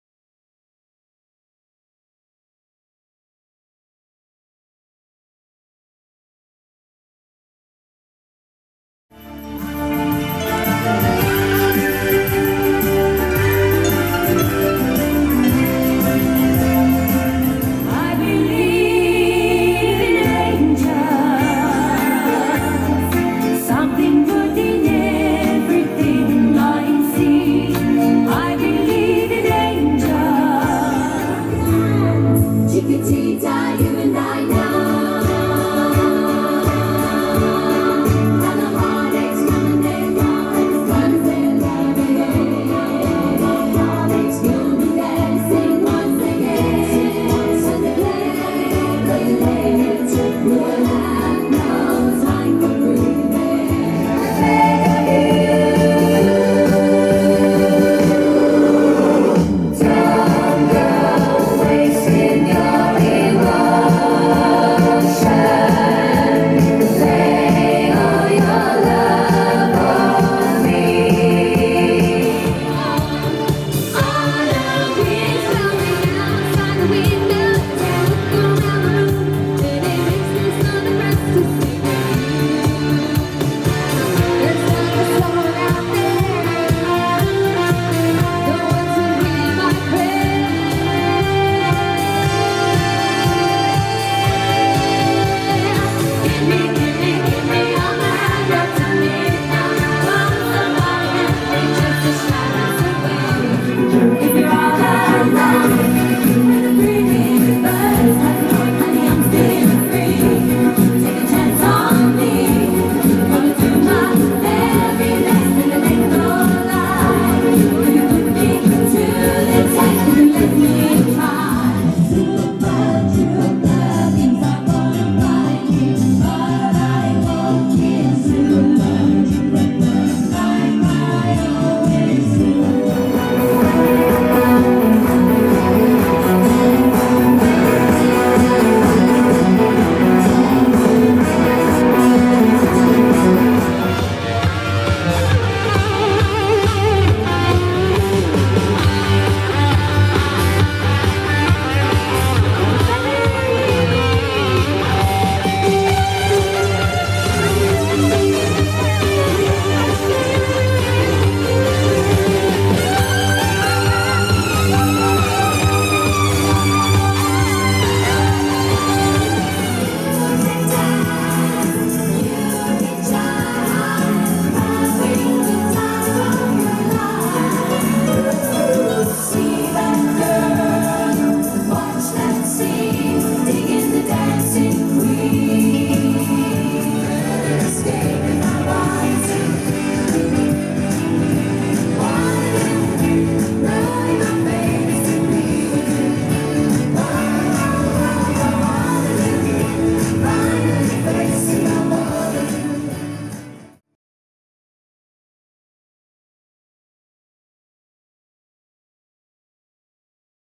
Time Off Festival ABBA
Time Off Festival come to East Bysshe, Lingfield.
Sunday 1st September is family day, come and see ABBA Chique, the HOTTEST tribute to ABBA perform in the afternoon.